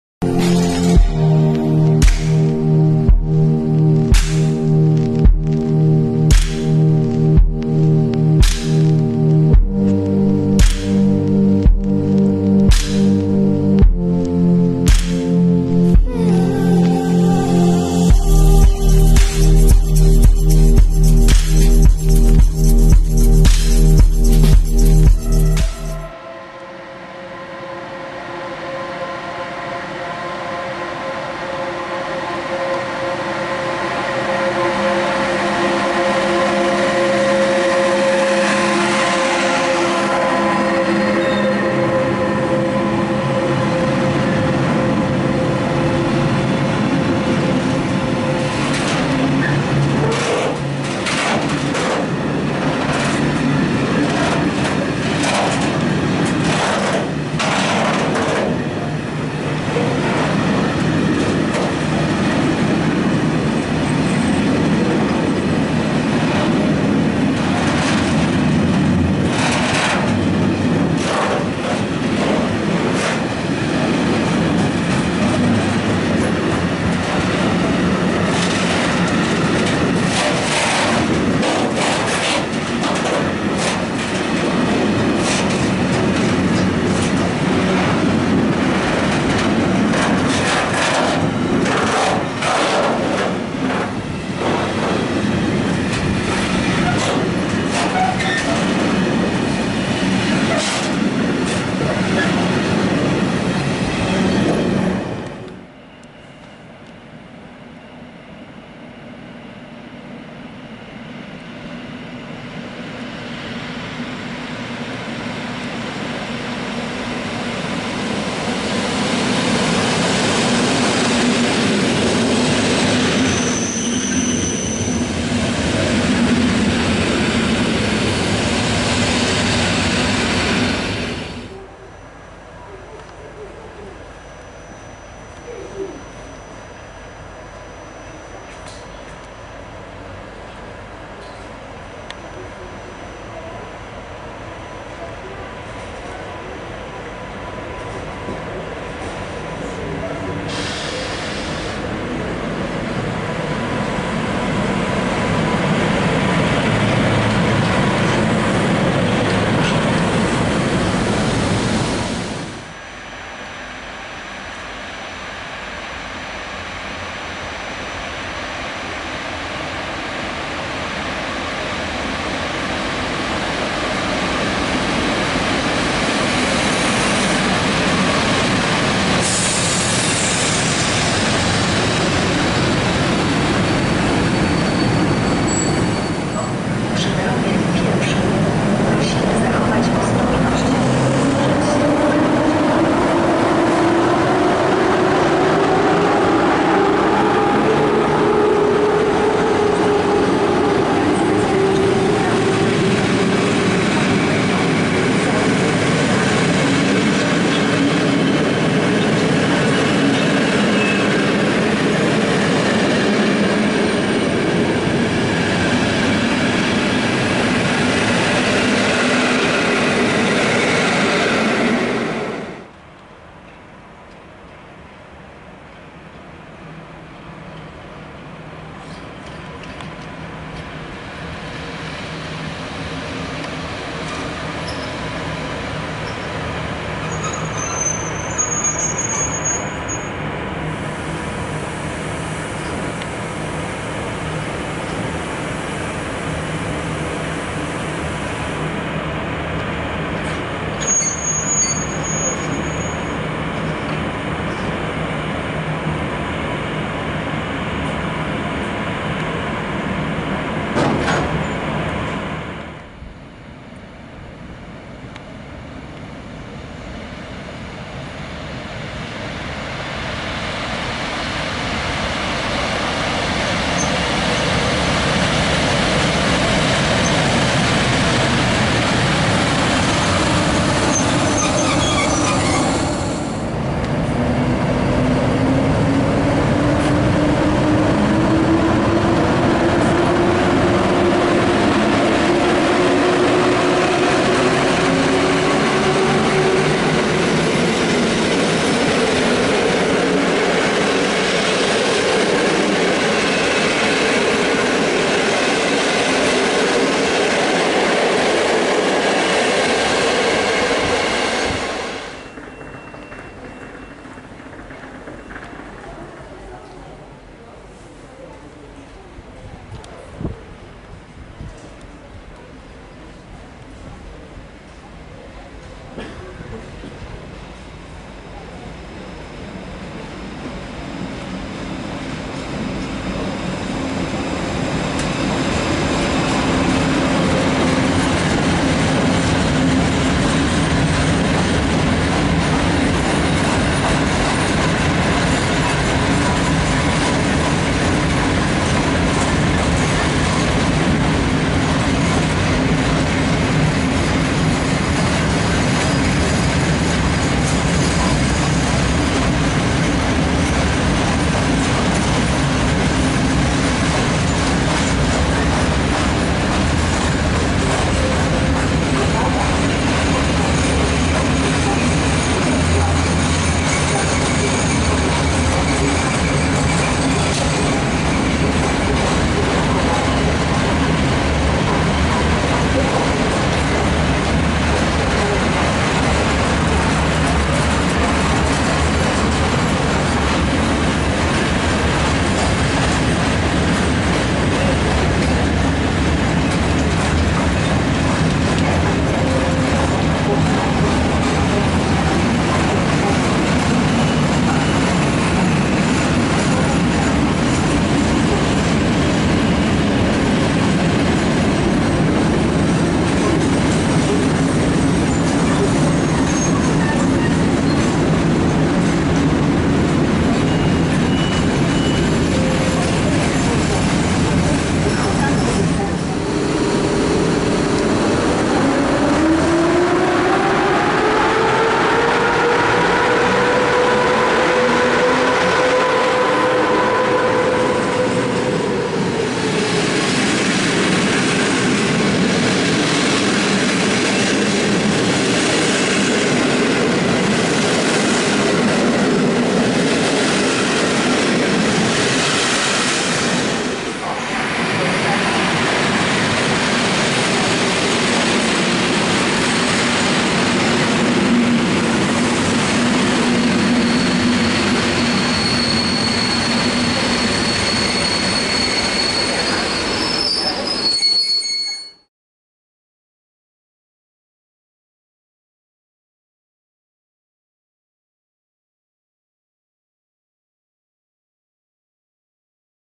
Pociągi na stacji Lubliniec TLK Orzeszkowa z EP05 Gama i nie tylko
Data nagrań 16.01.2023

00:00-Intro kanału

00:26-111Ed-037 z wagonami krytimi kierunku Olesna Śląskiego

01:46-EN57KM jako S8 relacji Katowice Lubliniec. @kolejowyukasz4086 na pokładzie

02:12-manewry ET22-903

02:43-EP07-1029 z IC Osterwa relacji Szczecin główny Kraków główny

03:07-Odjazd IC Osterwa w kierunku Olesna Śląskiego

03:43-ET22-903 podjeżdża pod skład węglarek

04:23-Przyjazd Duetu EP05-23 EP08-009 z TLK Orzeszkowa

04:43-Odjazd EP05-23 EP08-009 w kierunku Częstochowy Stradom

05:13-111Ed-017 z gruszkami w kierunku Kochanowic

07:07-EP07-1056 z IC Mehoffer w kierunku Opola głównego

07:34-Kibel Jako Regio Relacji Wrocław Główny Lubliniec kończy bieg

07:52-Zakończenie

pozdrawiam pasjonat kolei kolejowe filmy 2023